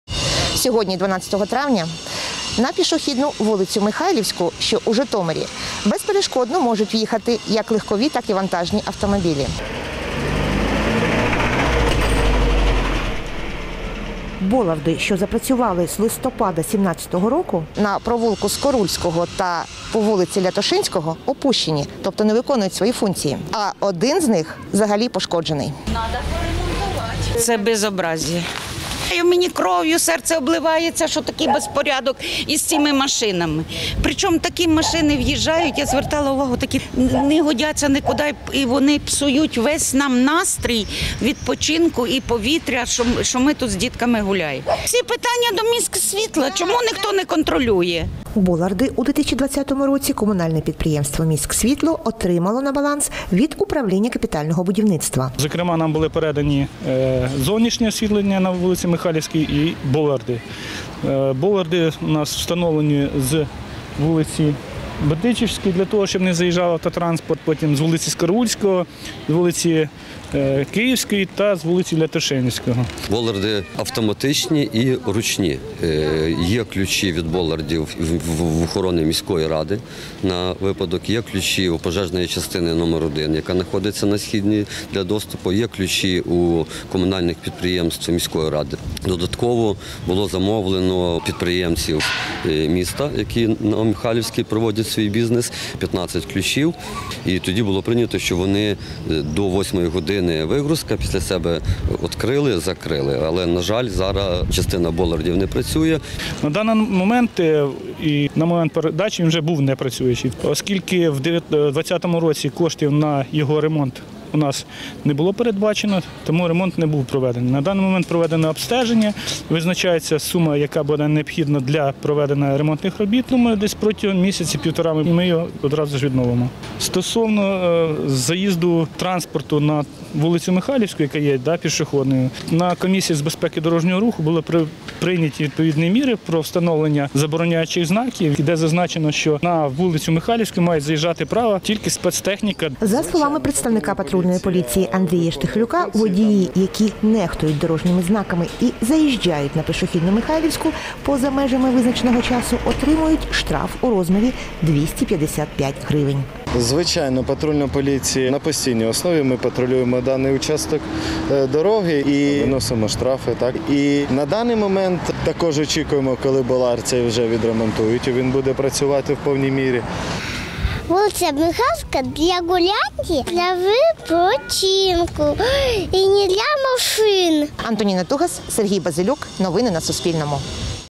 У Житомирі через опущені боларди пішохідною Михайлівською їздять машини
У Житомирі по пішохідній вулиці Михайлівській безперешкодно їздять автомобілі. Боларди, які мають перешкоджати заїзду – опущені, а один із них – зламаний.